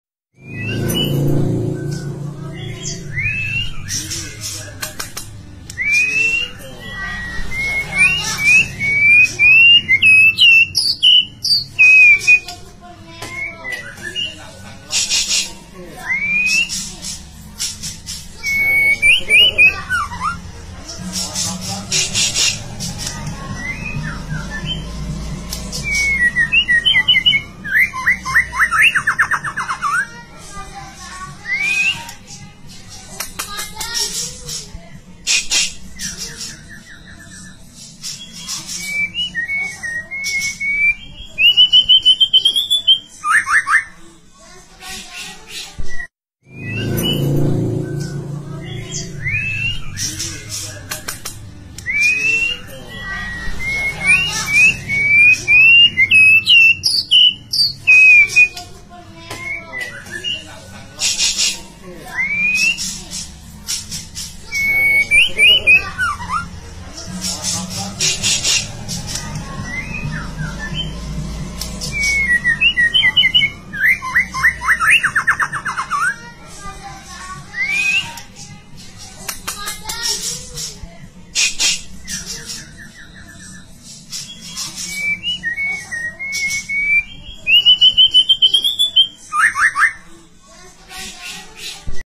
Suara Burung Murai Batu Betina GACOR
pancingan suara murai batu betina bikin jantan langsung bongkar,